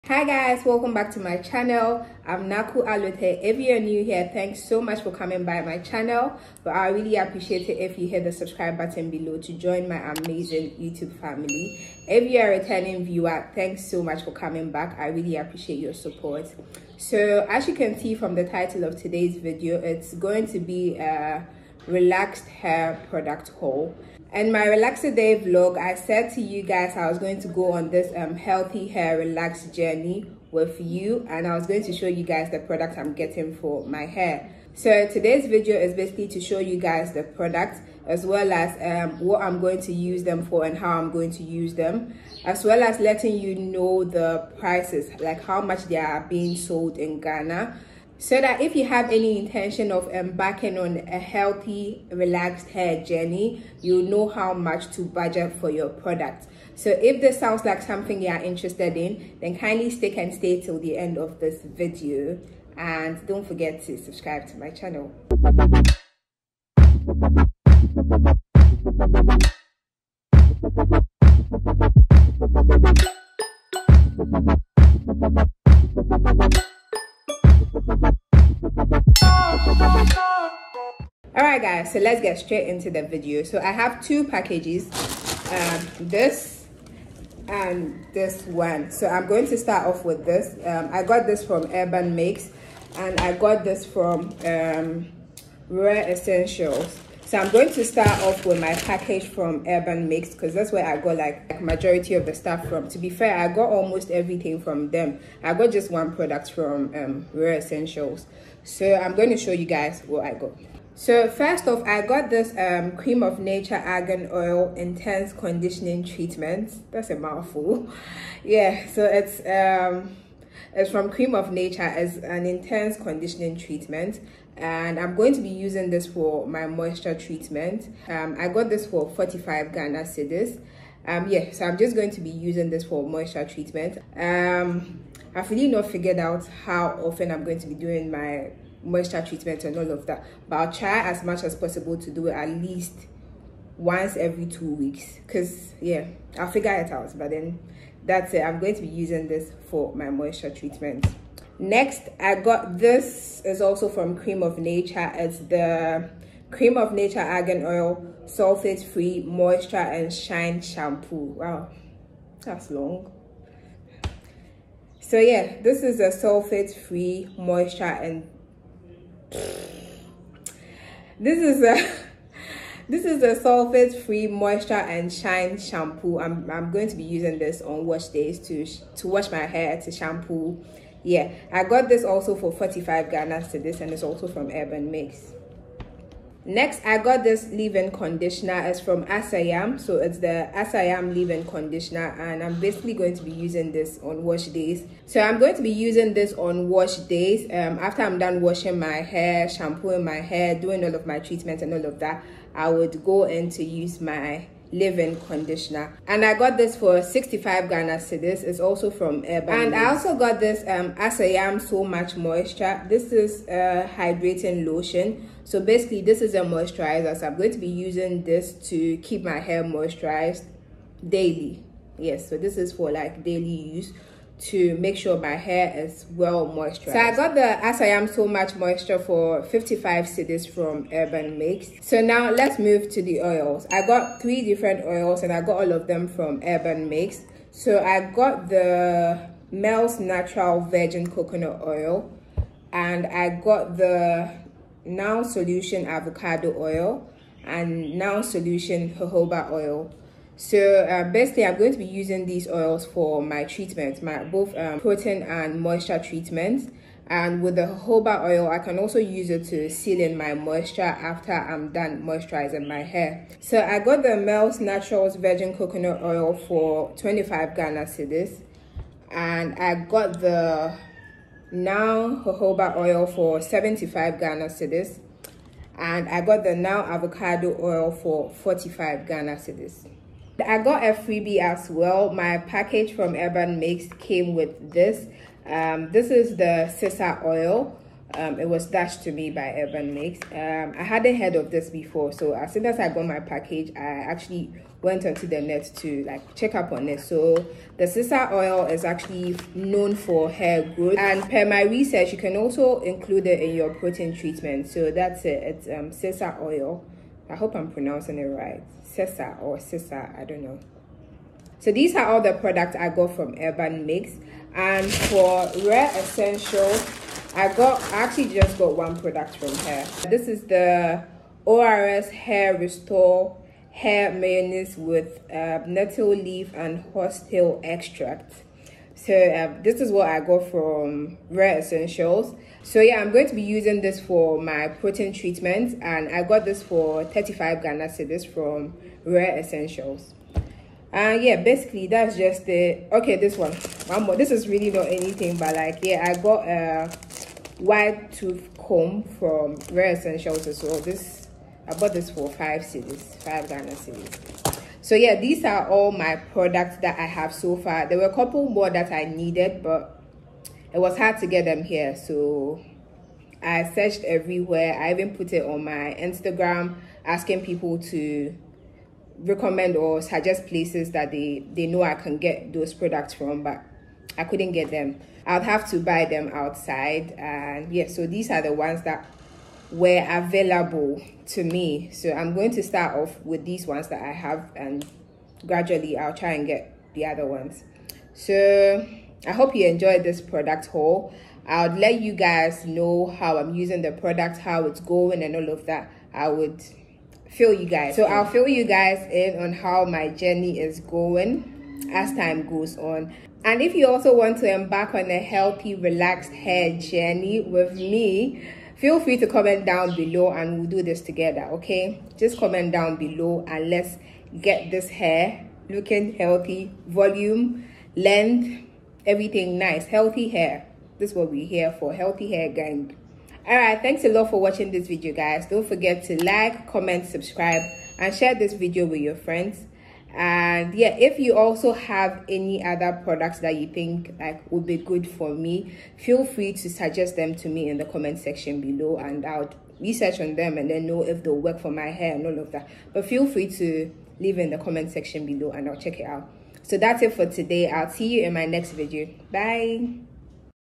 RELAXED HAIR PRODUCTS HAUL || PRODUCTS FOR HEALTHY RELAXED HAIR - ORS, CREME OF NATURE, AS I AM
Having the right relaxed hair products plays a very important role when embarking on a healthy relaxed hair journey.
Knowing the right relaxed hair products to use for your relaxed hair plays an important role in achieving a healthy relaxed hair; achieving good hair texture and long healthy hair

In this video, I will be sharing with you the products I will be using on my healthy relaxed hair journey to achieve a strong and healthy relaxed hair.
These products include products I will be using for protein treatment on my relaxed hair, products I will be using for moisture treatment on my relaxed hair as well as products I will be using to moisture my relaxed hair.

I also share with you what I’m going to use these products for as well as how I’m going to use these relaxed hair products as well as letting you know how much these products are being sold for in Ghana.

The relaxed hair products in this haul include
- Creme of Nature Argan Oil Intensive Conditioning Treatment
- Creme of Nature Sulfate-free Moisture & Shine Shampoo
- As I Am Leave In Conditioner
- As I Am So Much Moisture
- NOW Solutions Avocado  Oil
- NOW Solutions Jojoba Oil
- Mels Naturals Virgin Coconut Oil
- ORS  HaiRestore Hair Mayonnaise with Nettle Leaf & Horsetail Extract
- Sesa Oil

WHERE TO SHOP THESE PRODUCTS IN GHANA